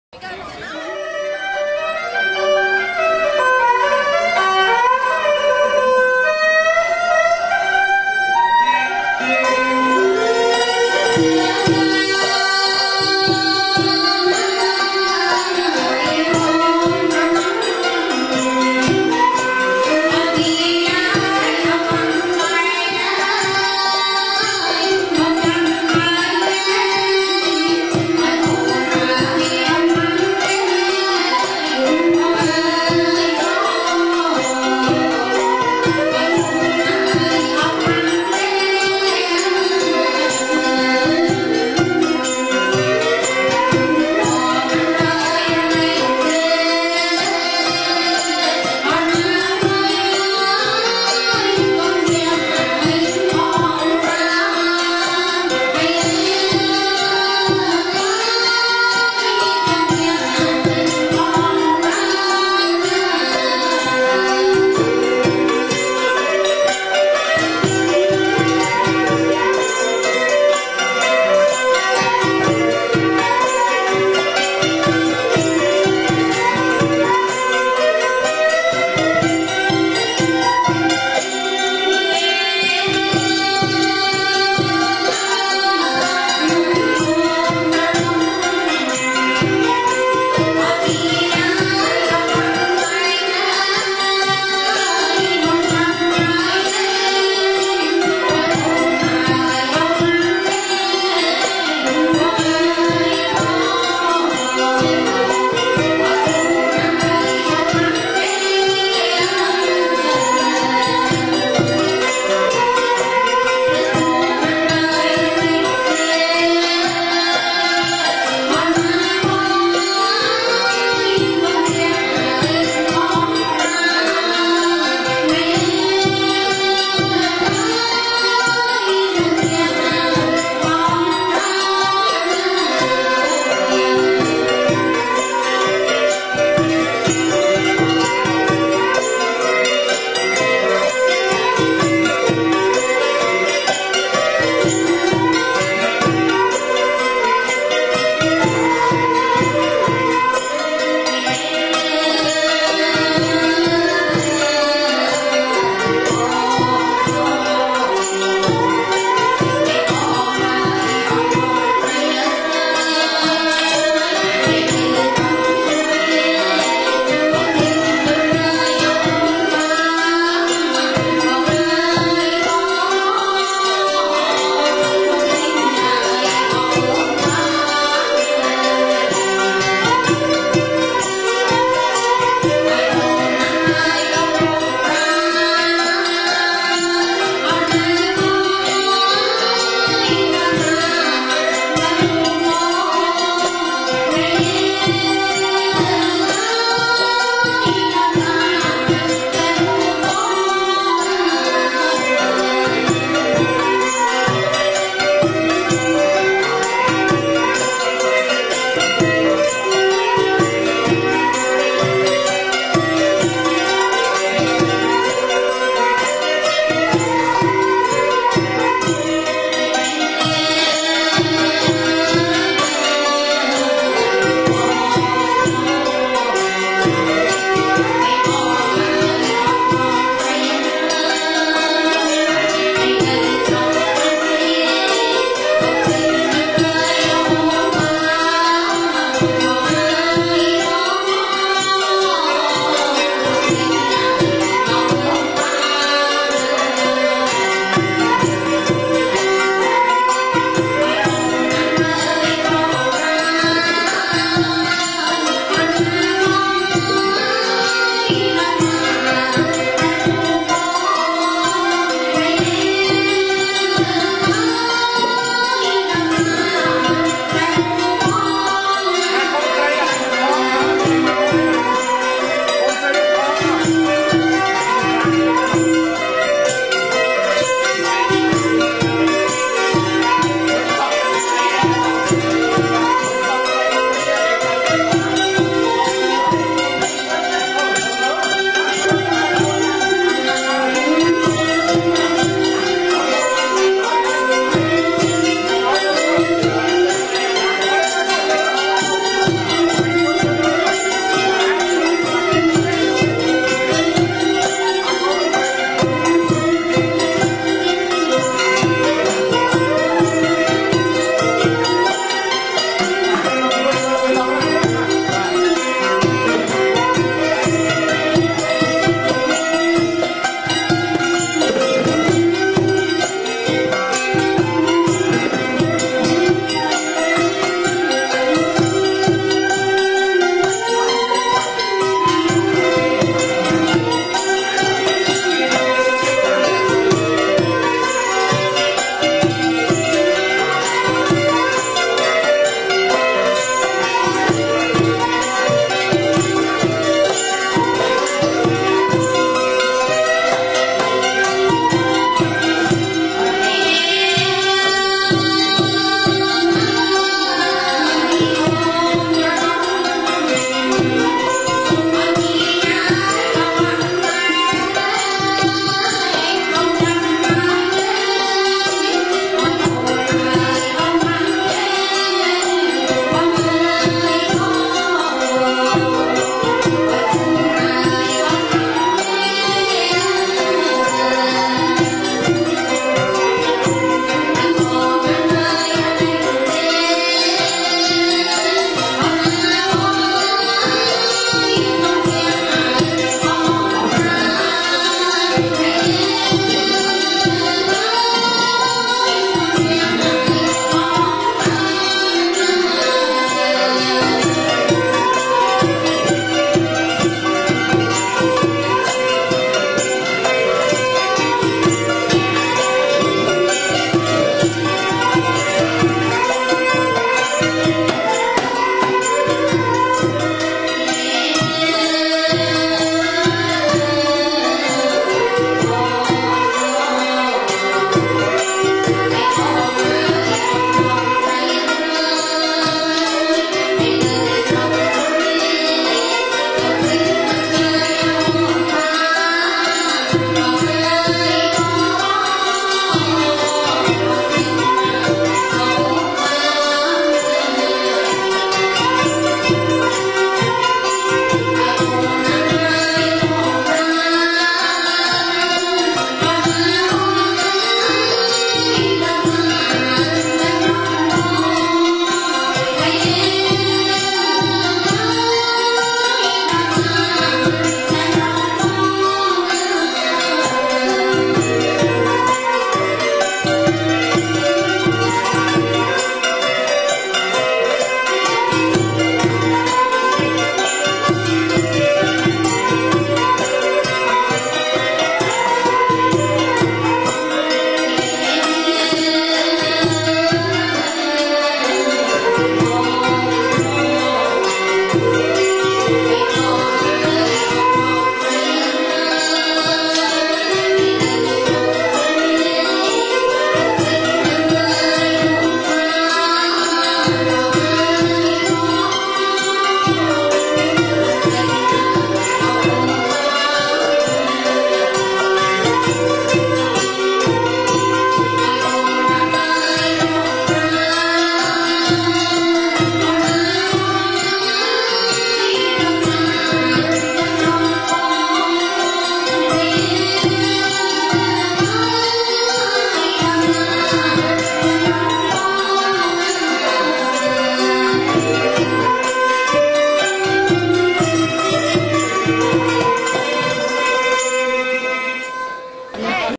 khmer wedding song1
this is one of the song during a khmer wedding.